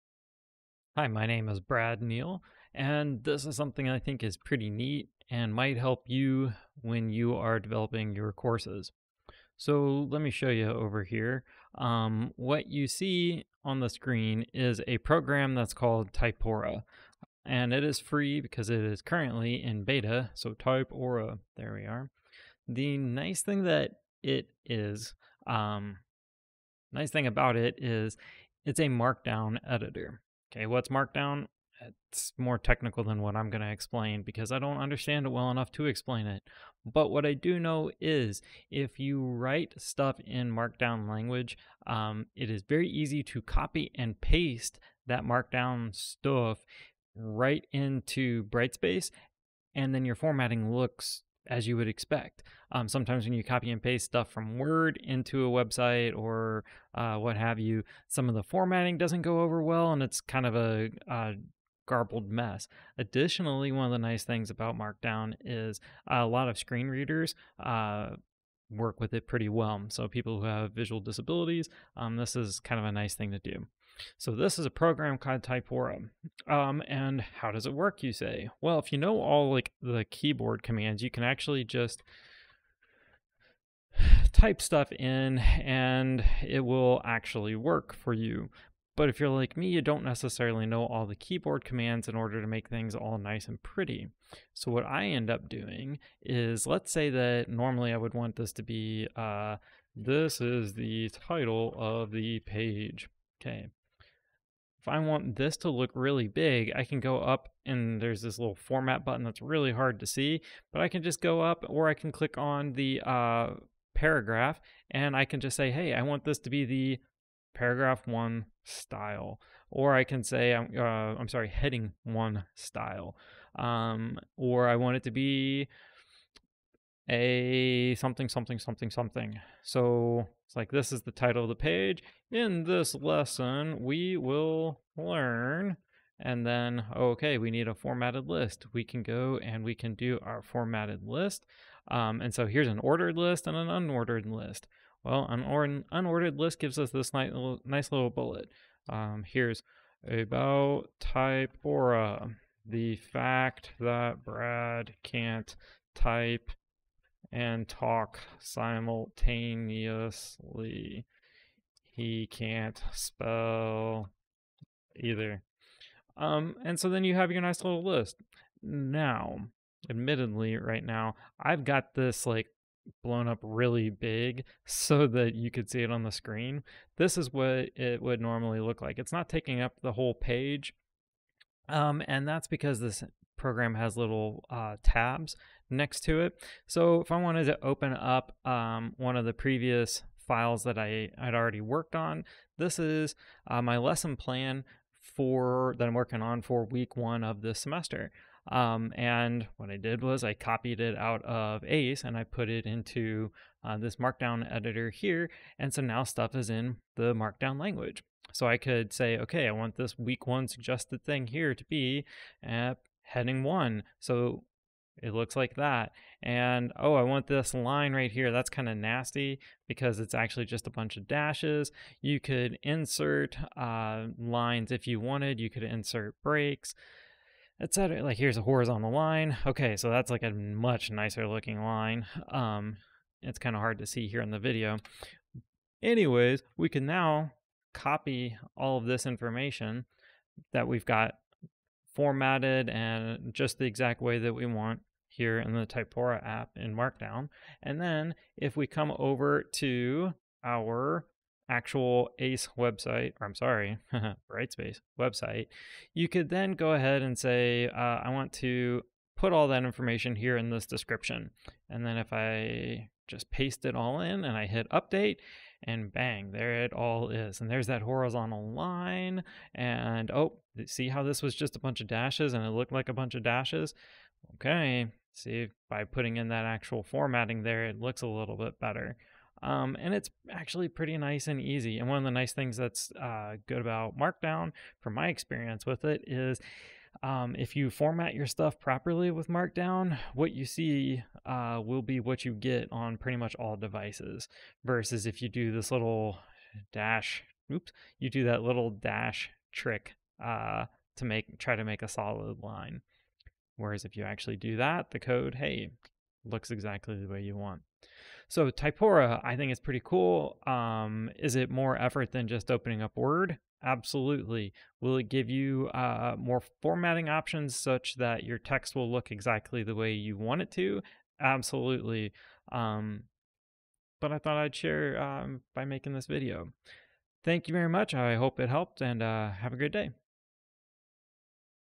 Things that are cool: Typora
This is a quick video about a piece of software called Typora that I'm using to make editing my Brightspace course sites easier.

Granted, it requires a person to know a little about markdown, but I assure you I know very little and can still use the software just with the drop down menu options.  Plus if you have a Mac with a touch bar, it's even easier.

Just in case you are wanting to dive off the deep end about markdown, here's a neat link that will definitely overwhelm all be the bravest of hearts.